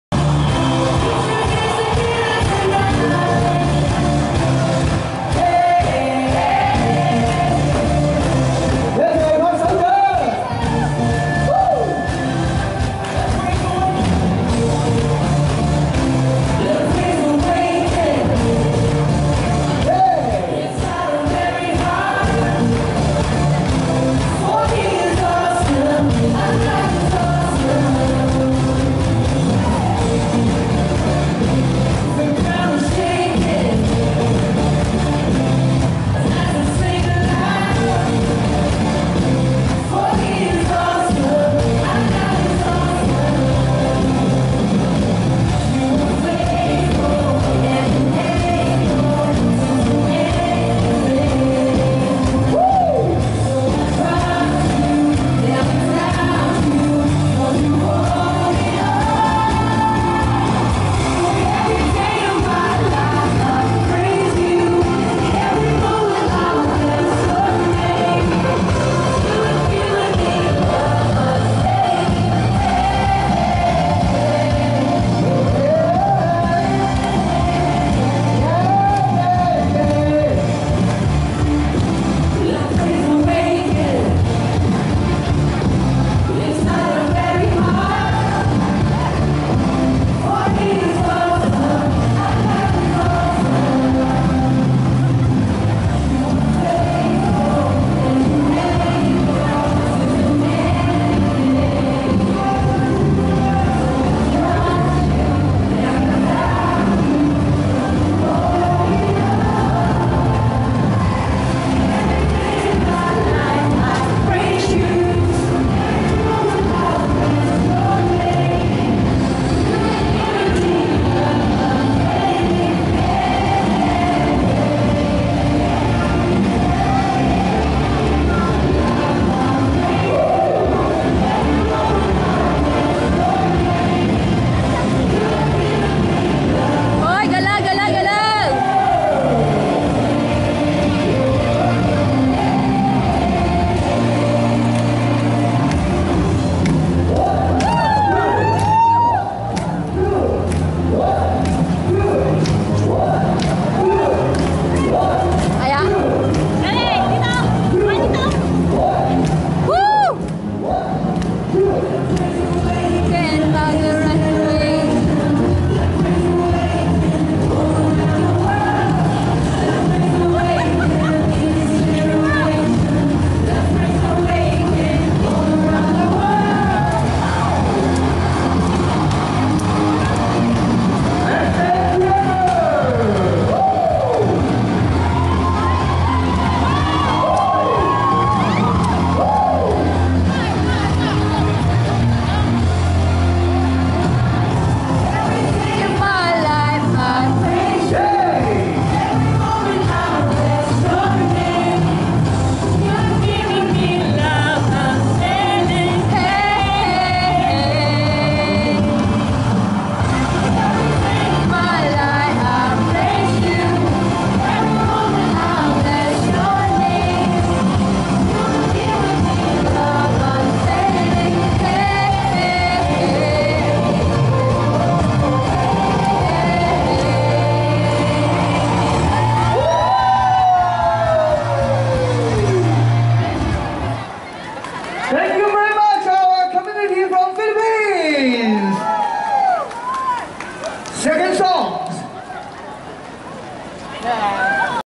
FEAST OF CHRIST THE KING CELEBRATION SAINT ANTHONY EVANGELIZATION ( 25-11-18)
Street dancing @saint Anthony